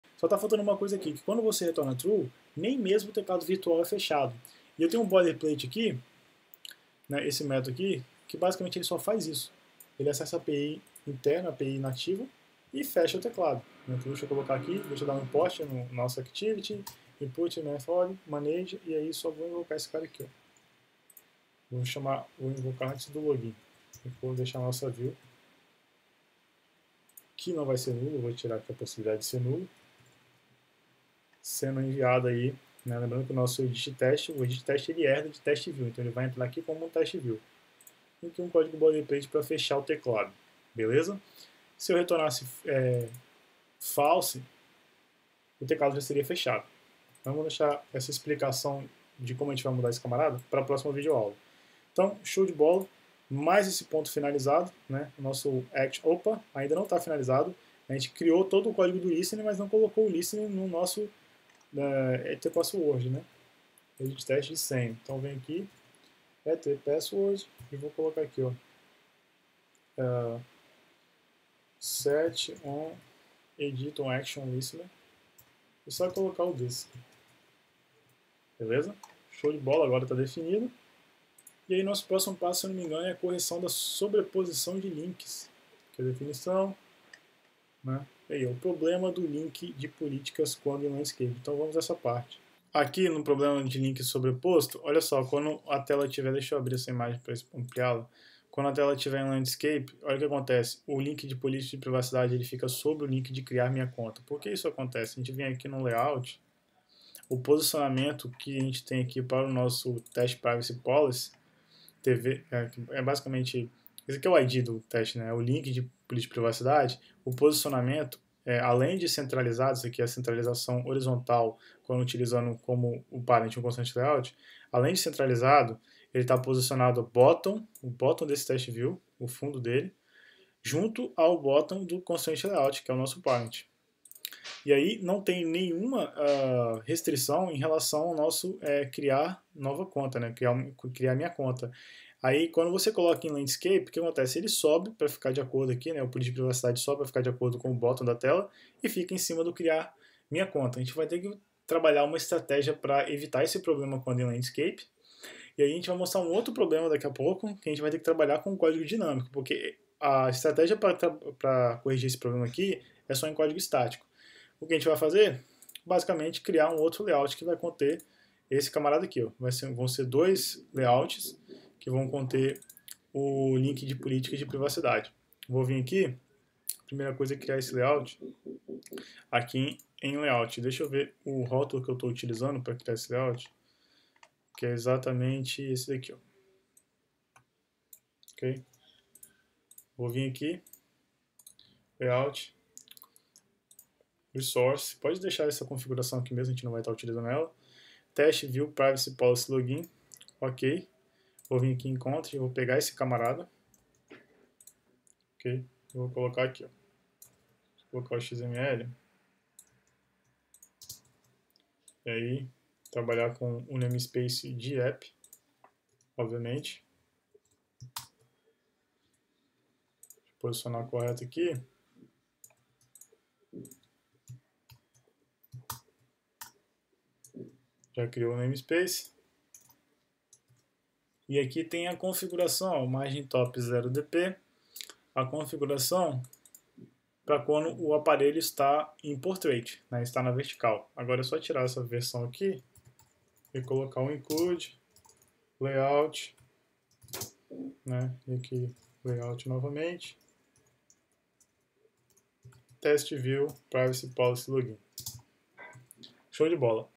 Login com ConstraintLayout e TextWatcher Para Validação - Android M-Commerce - PARTE 16
Nesta série de 20 vídeos vamos a construção da atividade de login do projeto Android de mobile-commerce, BlueShoes.

Devido ao tamanho dos códigos desta atividade, nesta aula vamos focar somente na construção dos códigos "brutos", posteriormente, em refatoração, vamos melhora-los.

📫 Para receber com prioridade os conteúdos exclusivos do blog, assine a lista de emails

Abraço.